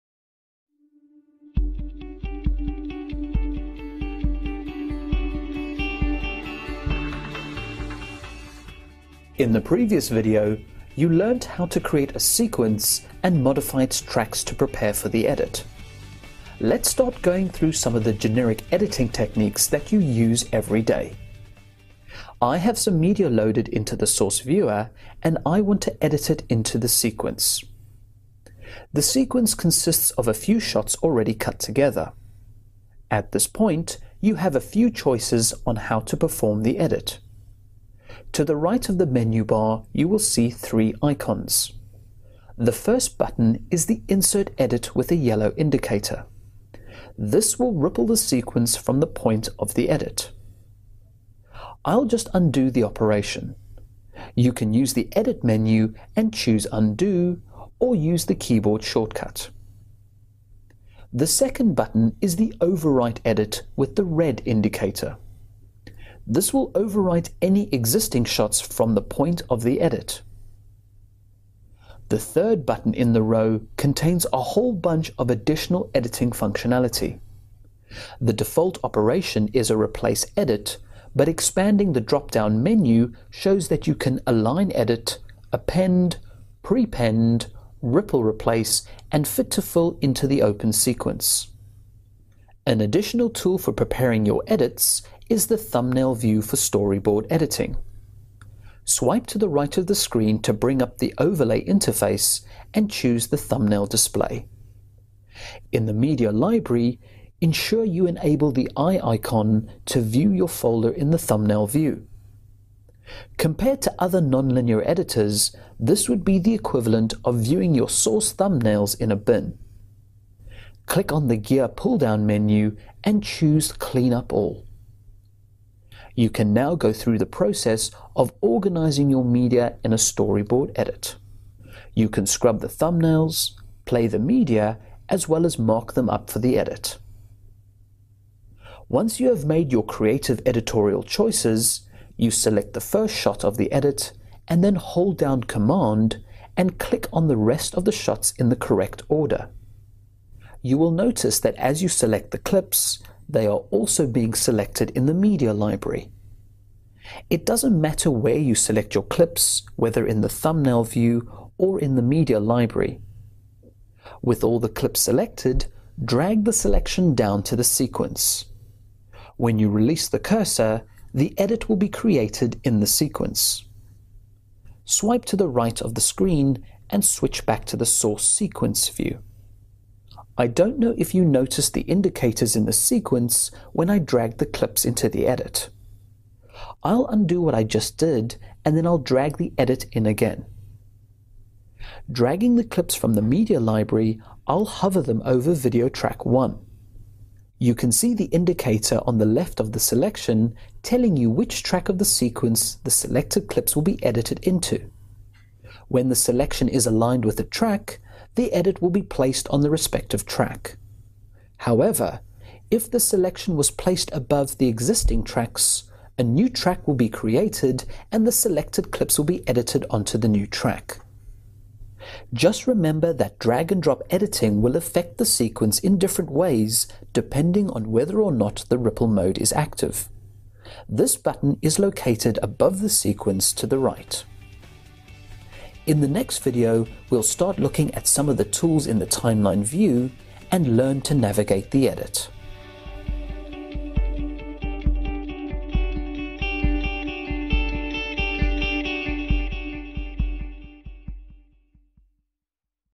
(Smoke 2013 教學影片) 10 Timeline Editing - 覆蓋、插入、拖放剪輯 (中文字幕)
中文字幕製作：堅達資訊